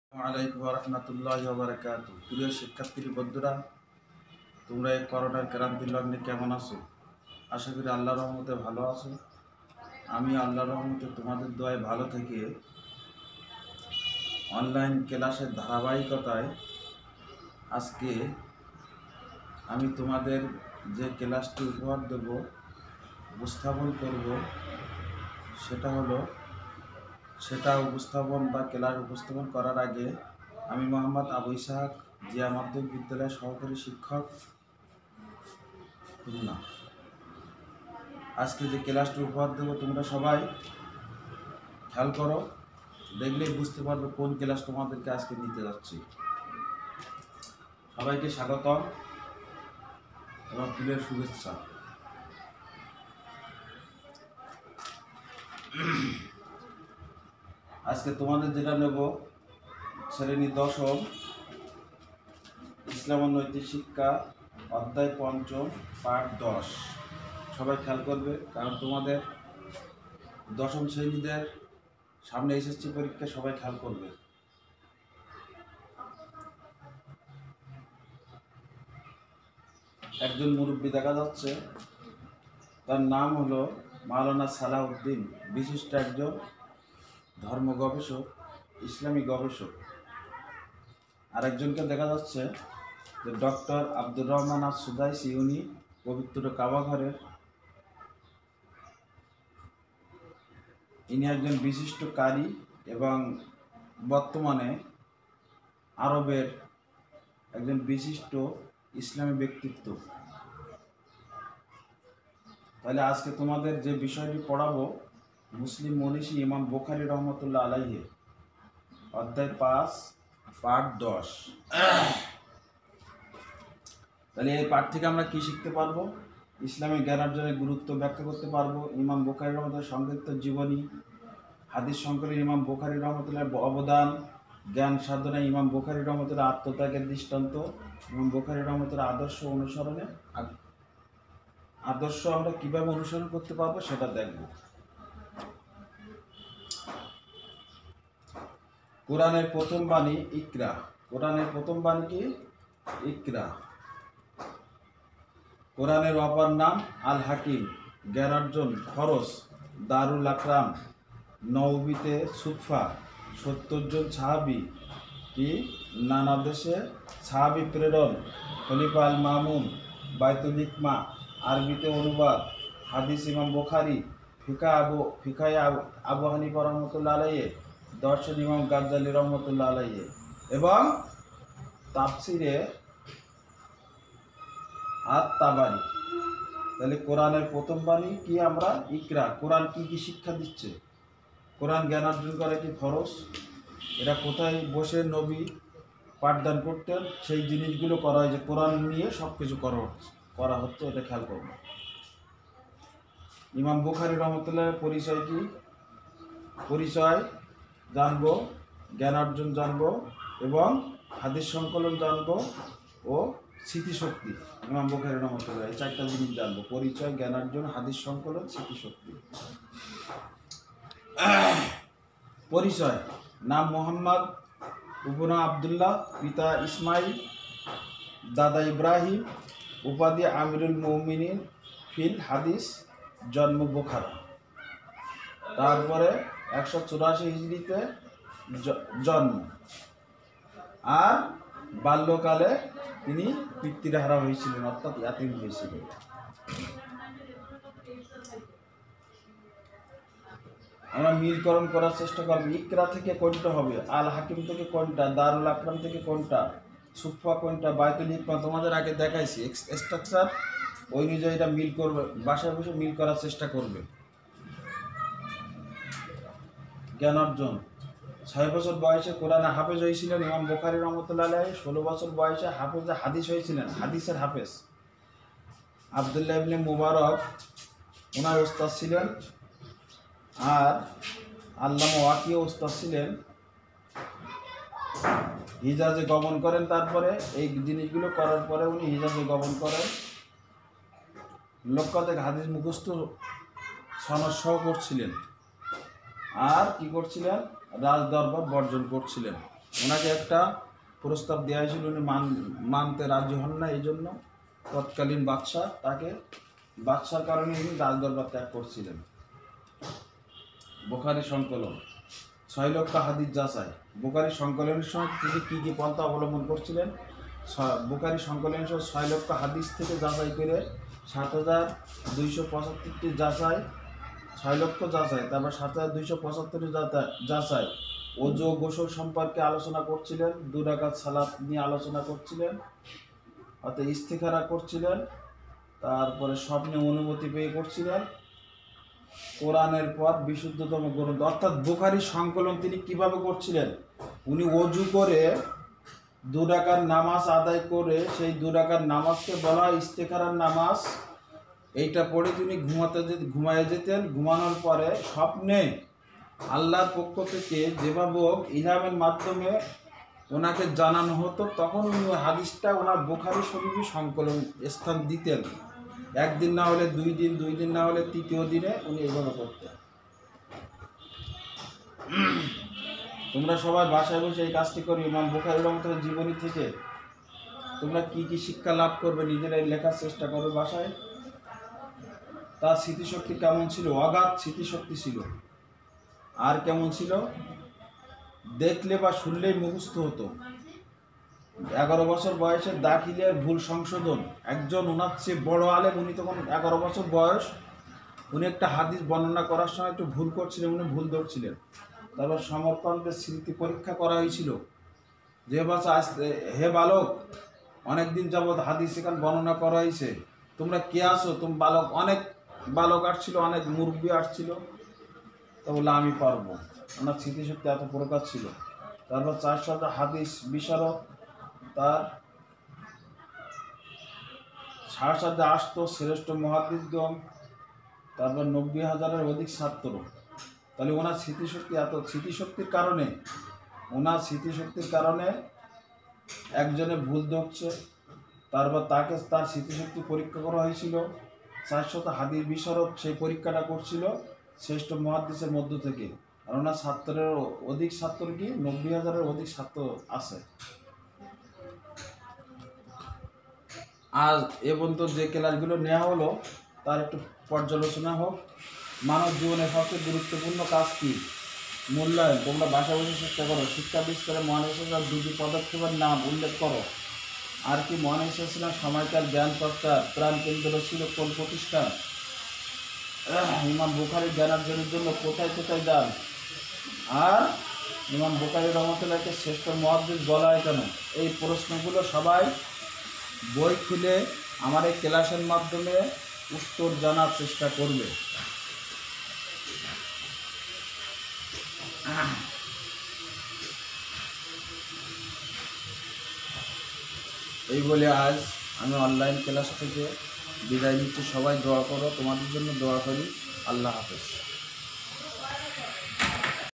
মোহাম্মদ আবু ইসহাক, সহঃ শিক্ষক, জিয়া মাধ্যমিক বিদ্যালয়, খুলনা।
১০ম শ্রেণি, ইসলাম ও নৈতিক শিক্ষা, অধ্যায়ঃ ৫ম, পাঠ-১০- ইমাম বুখারী (রঃ)